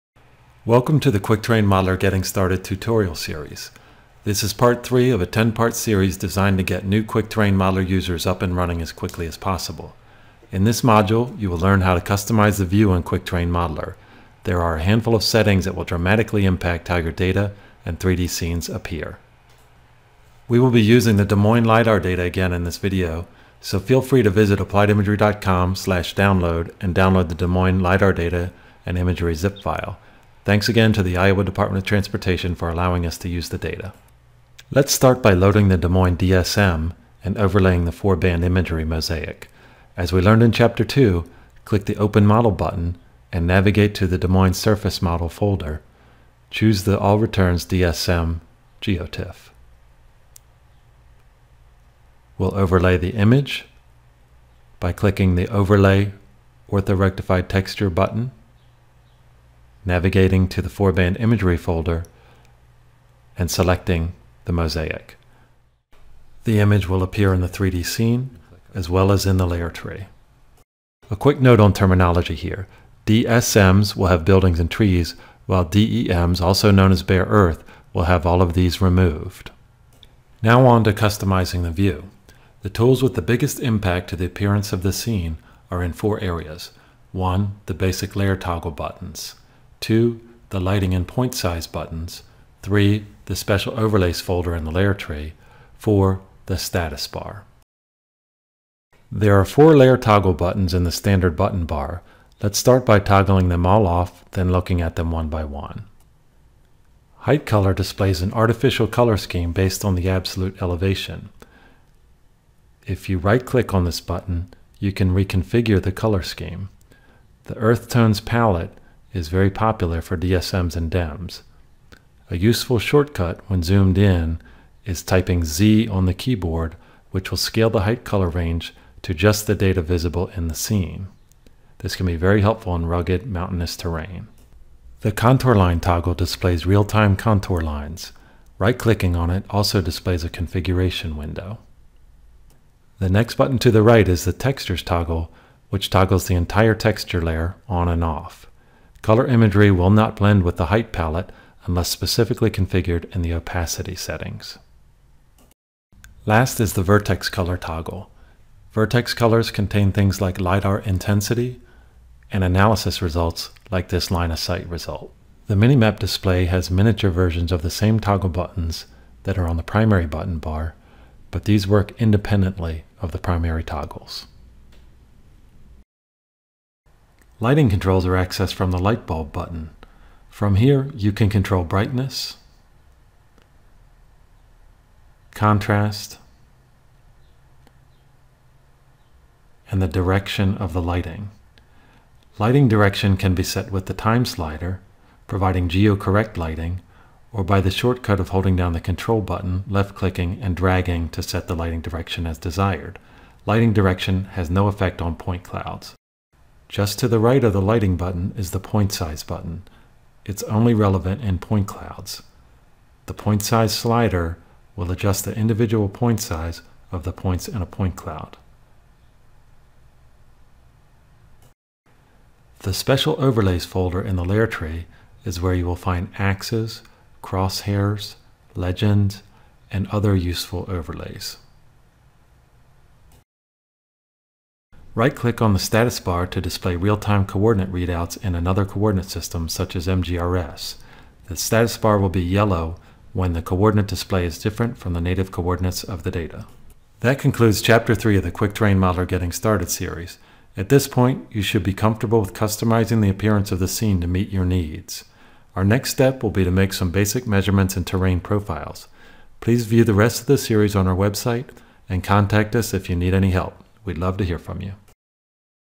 Quick Terrain Modeler Getting Started - 3 of 10 - Customizing the View
This is the third video in the Quick Terrain Modeler Getting Started Series.  It shows useful view settings for DEM's, DSM's, and Point Clouds.